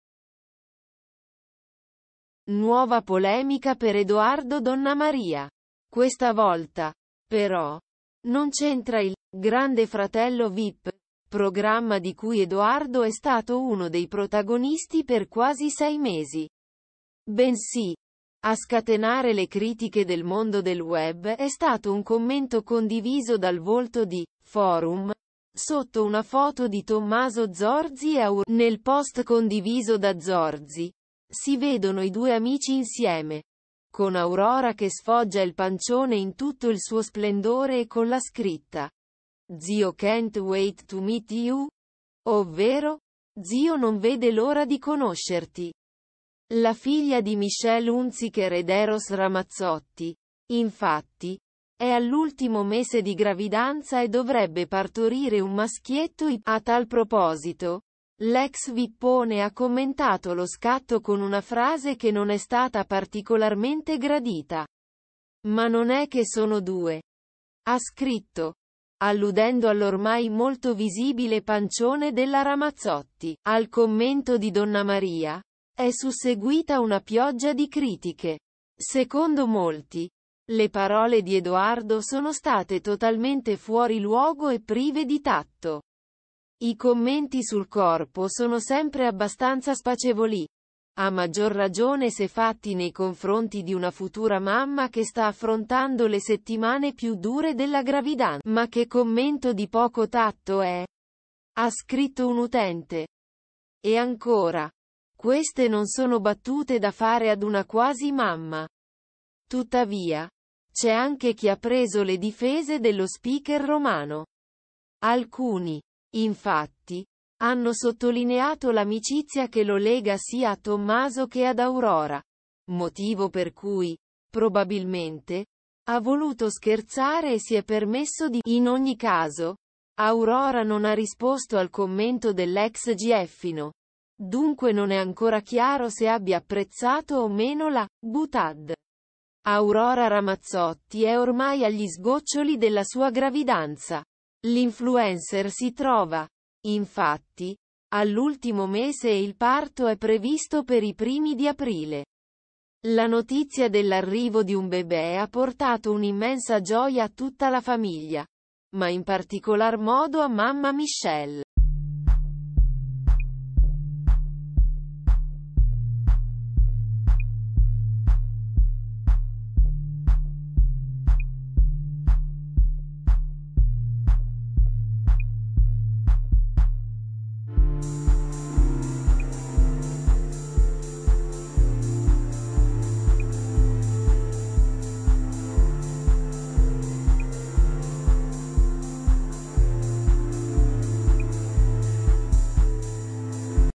Edoardo Donnamaria e la battuta sul pancione di Aurora Ramazzotti: caos, cosa ha detto
Pioggia di critiche per Edoardo Donnamaria a causa di un commento condiviso sotto una foto di Aurora Ramazzotti: cosa è successo